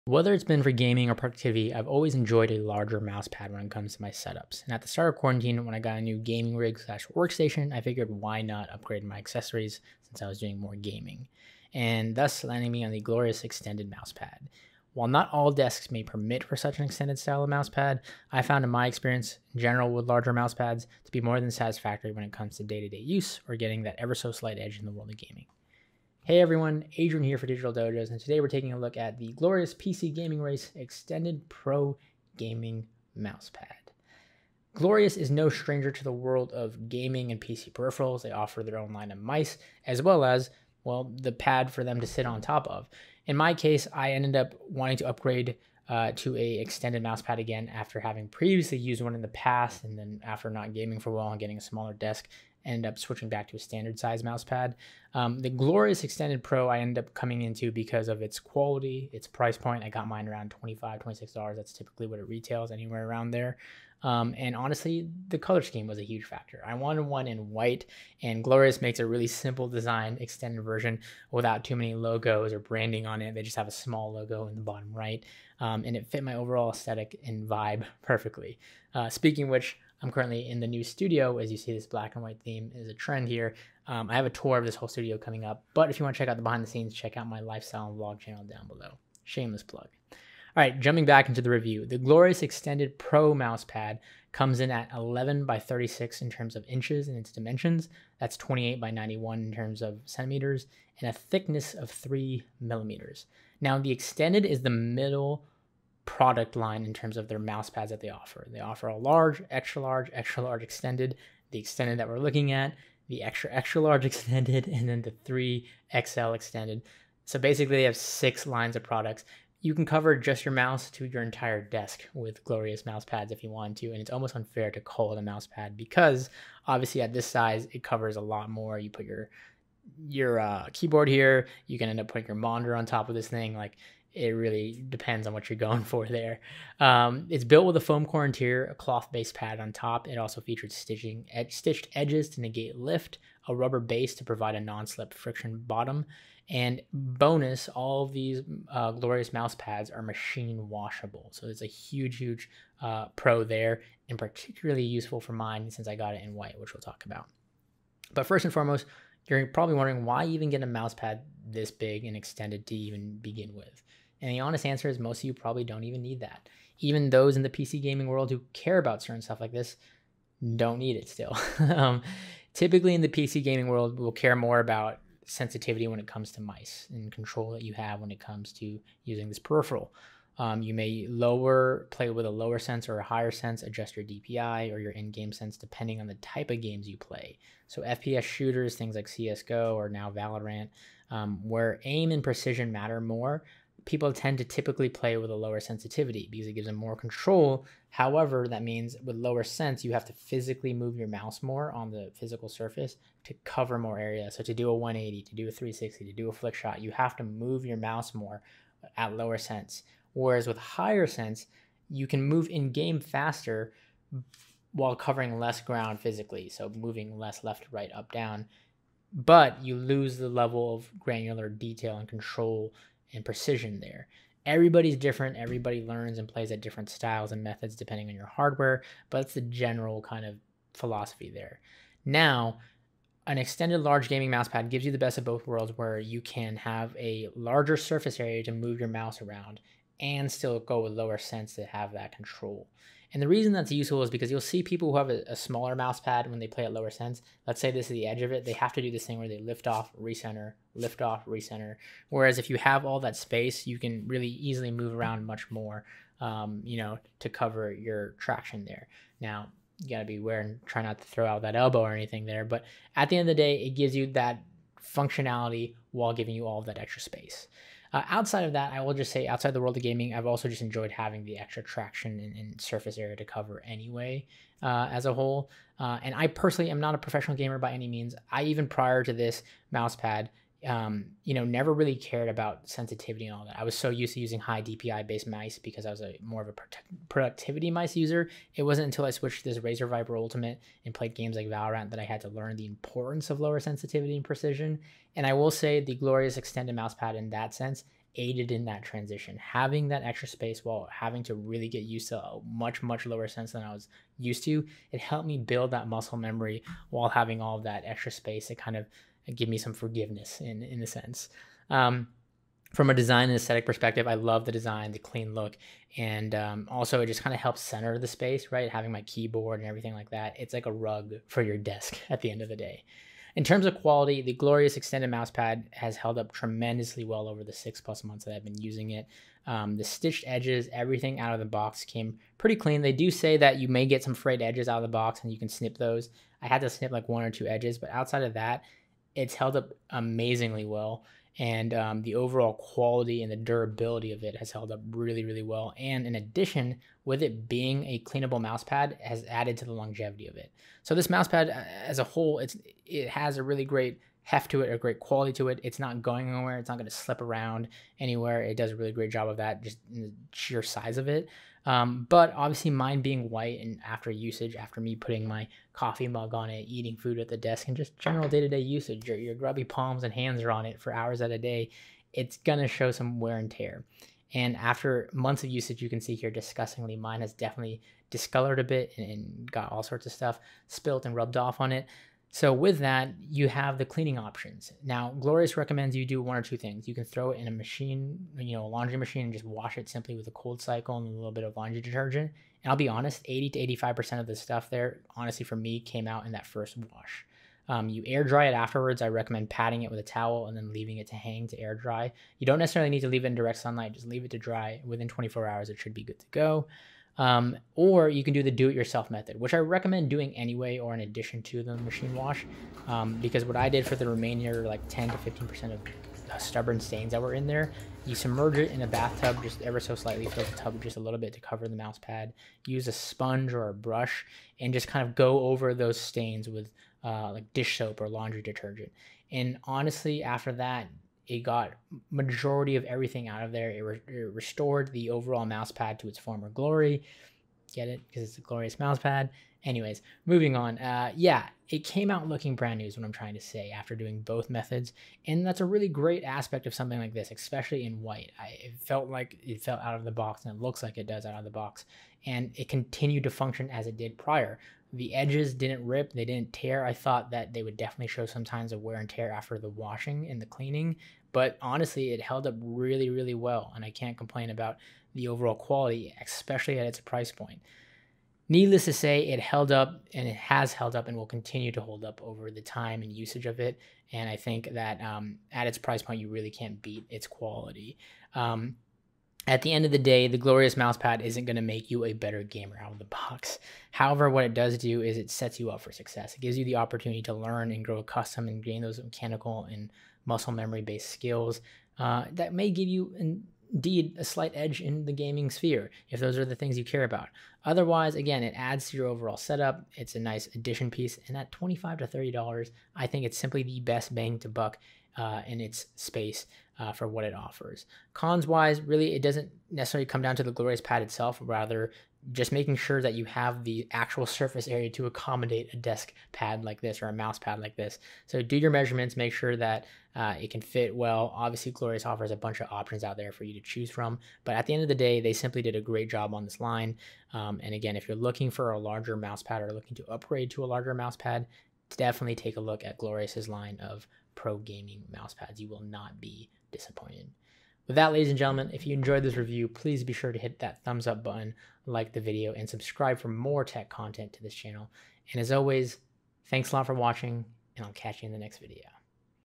Glorious Extended Mouse Pad Review. A whole lot of mouse pad.
A review of Glorious PC Gaming Race's Extended Mouse Pad.